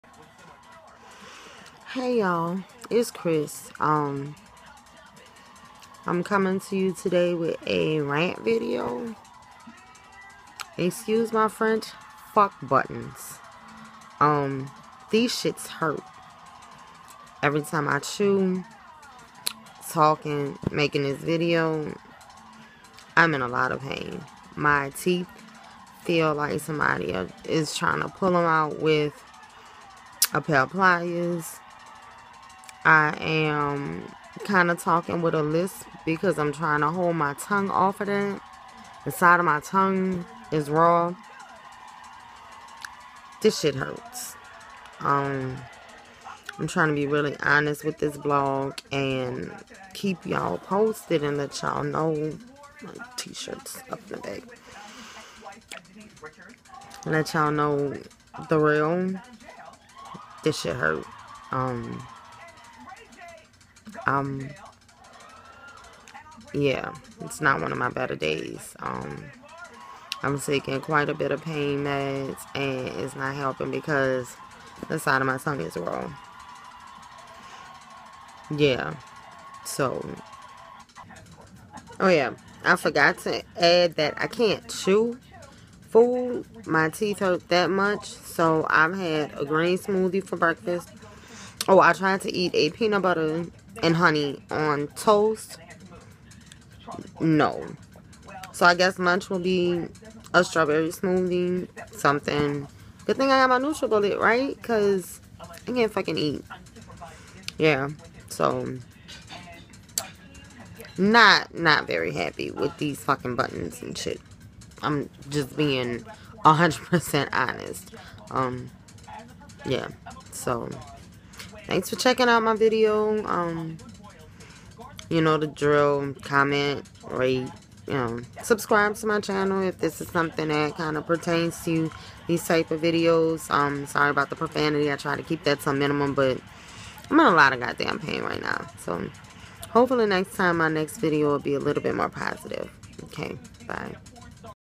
So yeah....FUCK buttons! (Adult braces update)
Update: rant vlog...these buttons) I'm assuming these are a type of TAD's...temporary anchorage devices) HURT! Dr Evans warned me that I wouldn't like these. I didn't know how much they'd hurt! They rub the sides of your tongue. A LOT! And it's raw. I already was dealing with the insides of my cheeks being raw...now this?! *sigh* this isn't a good day. But this wouldn't be an actual factual truthful blog if I left these parts out.